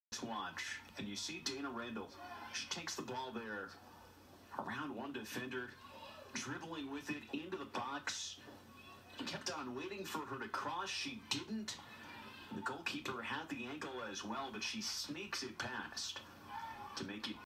Dayna scores in national soccer game
Ex-Tupton Hall School student Dayna Randall scores in the second round of the NAIA women's soccer tournament. Dayna plays forward for the Tennessee Wesleyan University Bulldogs women's team.